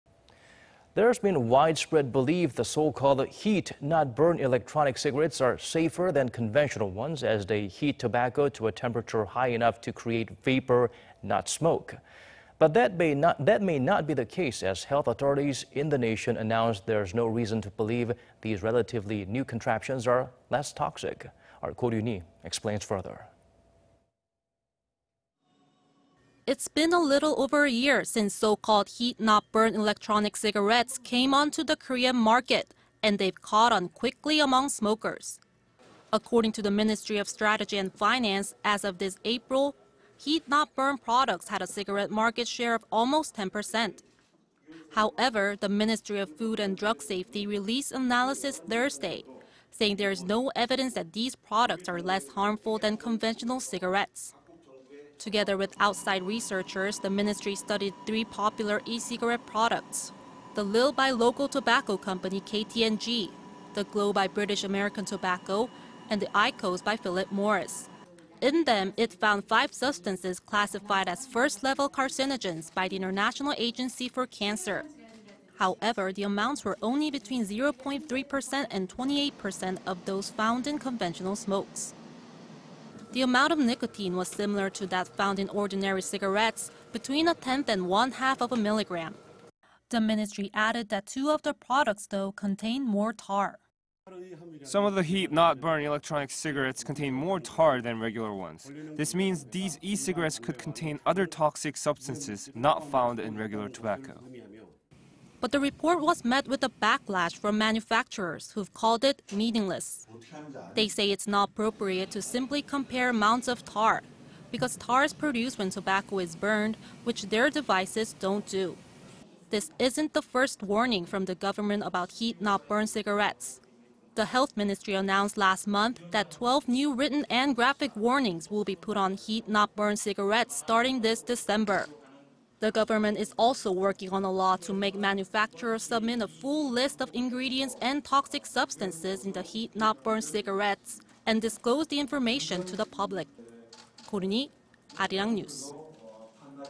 Five carcinogens found in "heat-not-burn" e-cigarettes
궐련형 전자담배에 1급 발암물질 5종...타르 일반담배보다 많아

  There's been widespread belief the so-called "heat-not-burn" electronic cigarettes are safer than conventional ones... as they heat tobacco to a temperature high enough to create vapor not smoke.
But that may not be true, as health authorities in the nation announced there's no reason to believe these relatively new contraptions are less toxic.
Our Ko Roon-hee explains further.
  It's been a little over a year since so-called "heat-not-burn" electronic cigarettes came onto the Korean market, and they've caught on quickly among smokers.
According to the Ministry of Strategy and Finance, as of this April, heat-not-burn products had a cigarette market share of almost 10 percent.
 However, the Ministry of Food and Drug Safety released analysis Thursday... saying there is no evidence that these products are less harmful than conventional cigarettes.
Together with outside researchers, the ministry studied three popular e-cigarette products -- the Lil by local tobacco company KT&G, the Glo by British American Tobacco, and the iQOS by Philip Morris.
  In them, it found five substances classified as five substances classified as "first level carcinogens" by the International Agency for Cancer.
However, the amounts were only between zero-point-3 percent and 28 percent of those found in conventional smokes.
 The amount of nicotine was similar to that found in ordinary cigarettes -- between a tenth and one half of a milligram.
The ministry added that two of the products, though, contained more tar.

   "Some of the heat-not-burn electronic cigarettes contain more tar than regular ones.
This means these e-cigarettes could contain other toxic substances...not found in regular tobacco."
  But the report was met with a backlash from manufacturers, who've called it meaningless.
They say it's not appropriate to simply compare amounts of tar... because tar is produced when tobacco is burned, which their devices don't do.
  This isn't the first warning from the government about heat-not-burn cigarettes.
The health ministry announced last month that twelve new written and graphic warnings will be put on heat-not-burn cigarettes starting this December.
  The government is also working on a law to make manufacturers submit a full list of the ingredients and toxic substances in the heat-not-burn cigarettes and disclose the information to the public.
Ko Roon-hee, Arirang News.